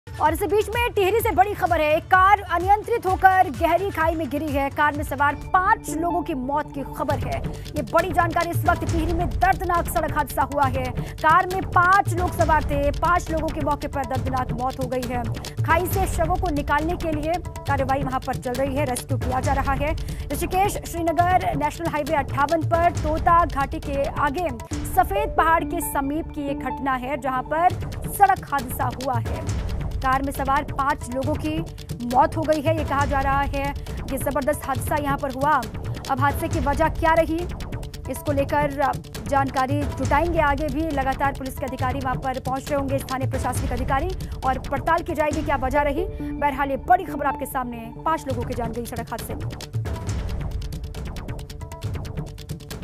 BREAKING: ऋषिकेश-श्रीनगर एनएच- 58 पर कार दुर्घटना, खाई में गिरी कार..5 लोगों की मौत
टिहरी से एक बड़ी खबर आ रही है। एक कार अनियंत्रित होकर गहरी खाई में गिरी। कार में सवार 5 लोगो की मौके पर मौत हो गई है। खाई से शवों को निकाला जा रहा है। ऋषिकेश-श्रीनगर एनएच- 58 पर तोता घाटी से आगे सफेद पहाड़ के समीप की घटना।